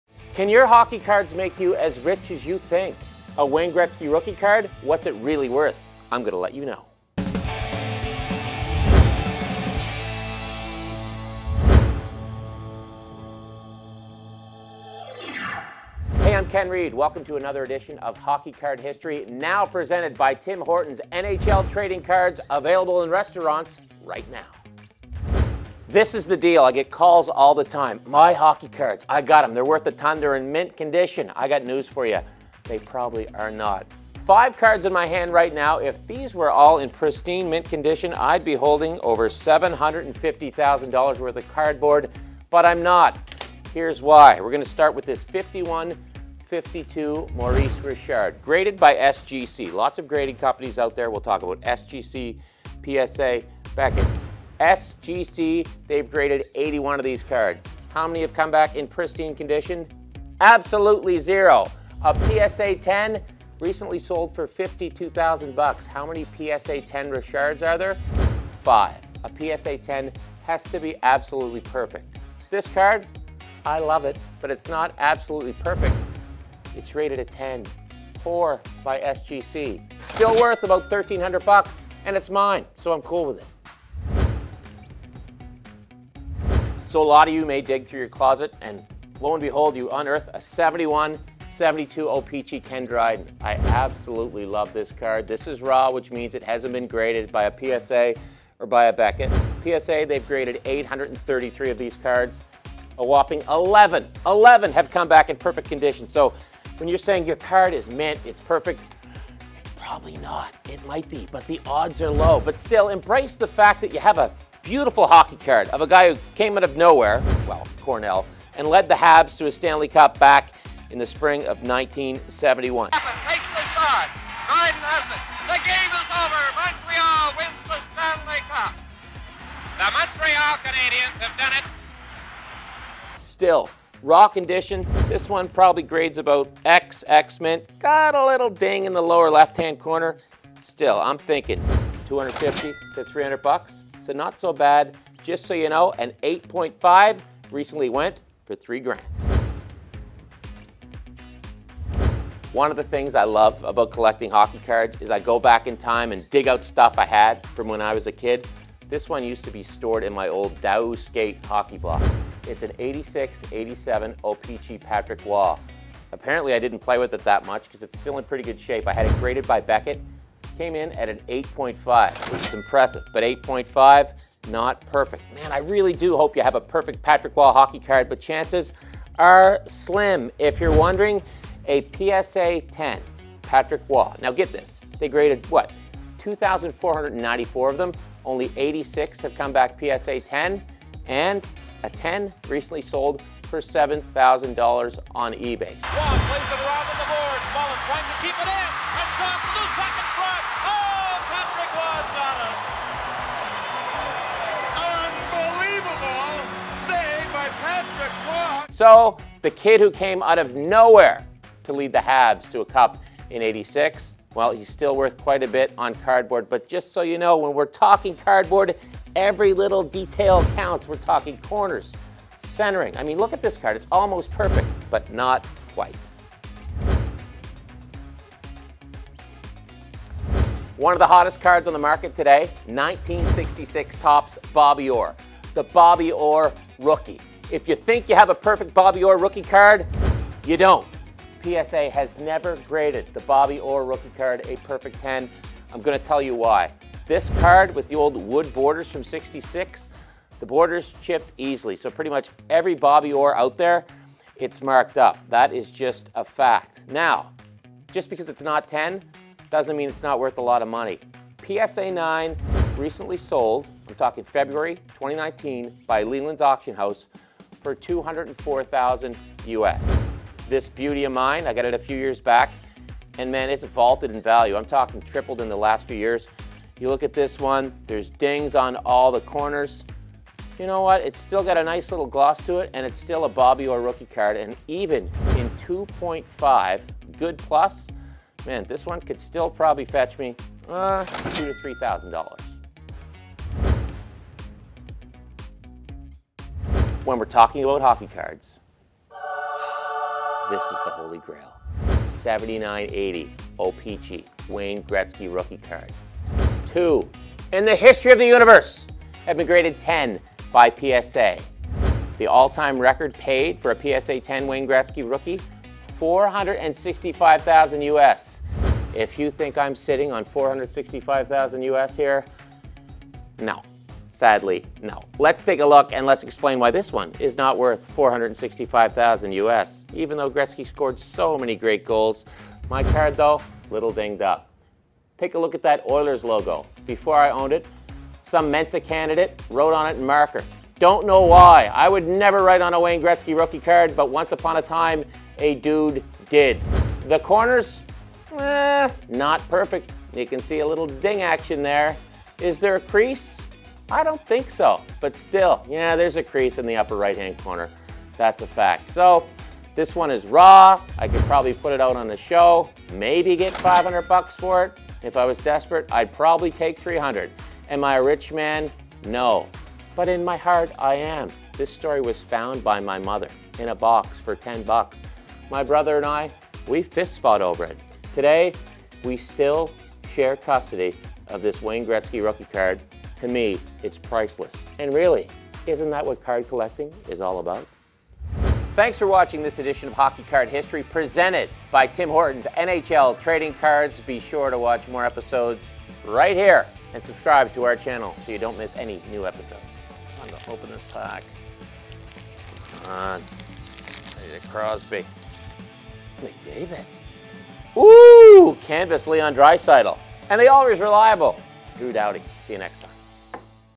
Can Your Hockey Cards Make You Rich? | Hockey Card History
In the latest edition of Hockey Card History, Ken Reid takes us through five of the most expensive hockey cards in world and why the condition of the card is a massivly important factor.